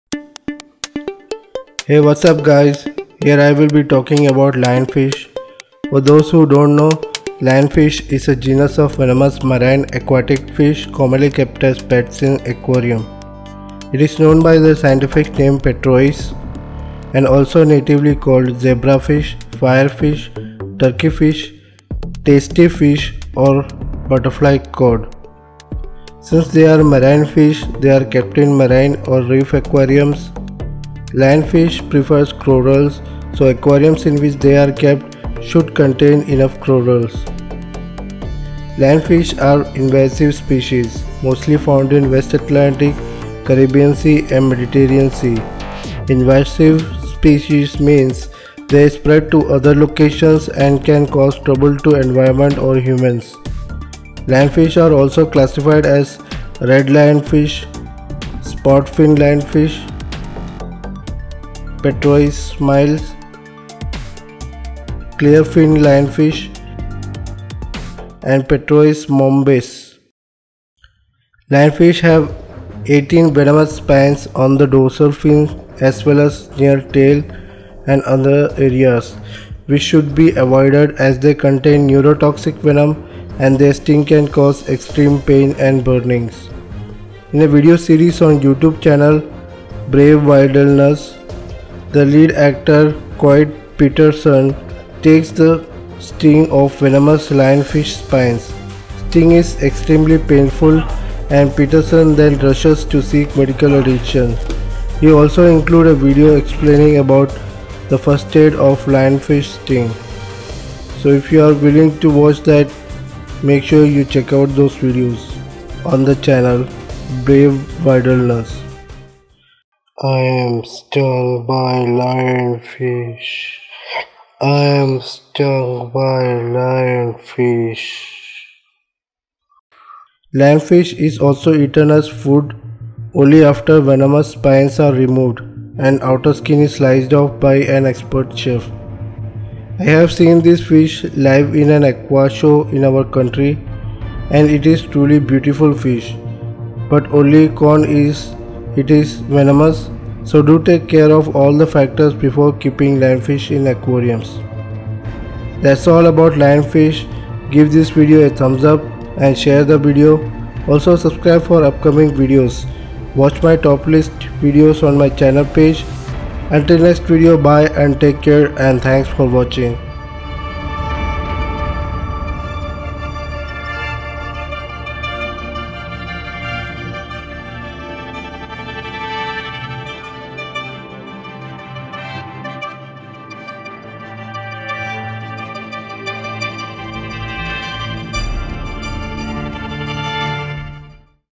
Lionfish Facts and Care Guide
Lionfish is a genus of venomous marine fish kept as pets in aquariums. They are also natively called zebrafish, firefish, turkeyfish, tastyfish or butterfly-cod. Since they are marine fish they are kept in marine or reef aquariums. Lion Fish prefer corals so aquariums in which they are kept should contain enough corals. Watch this video about facts and care guide about Lionfish

Connect me on Social Networks too

All music have been downloaded from YouTube Music Library.